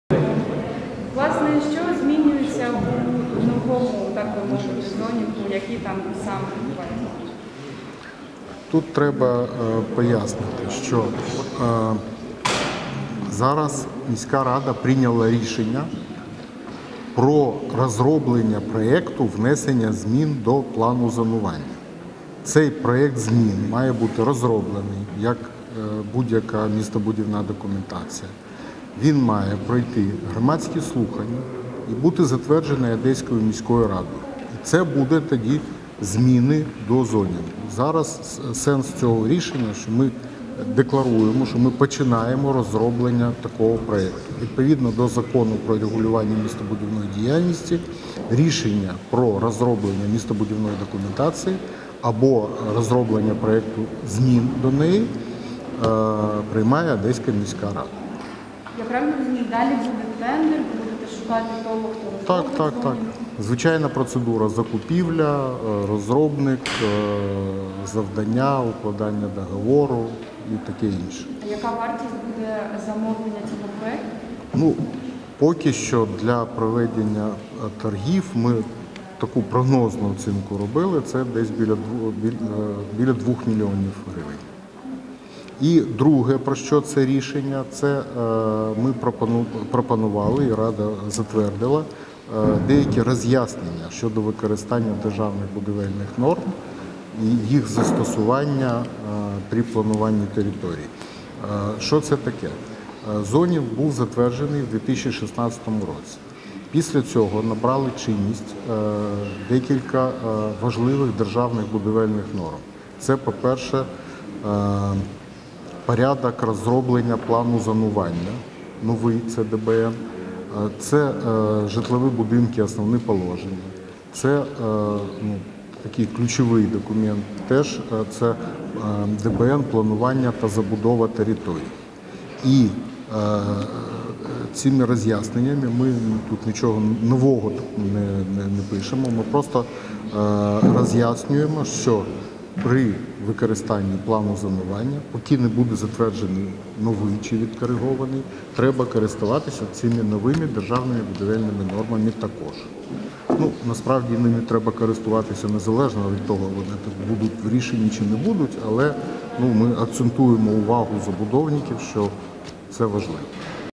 Про розробку проєкту змін в план зонінгу.
Коментар заступника директора департаменту архітектури та містобудування Одеської міської ради Марата Касімова.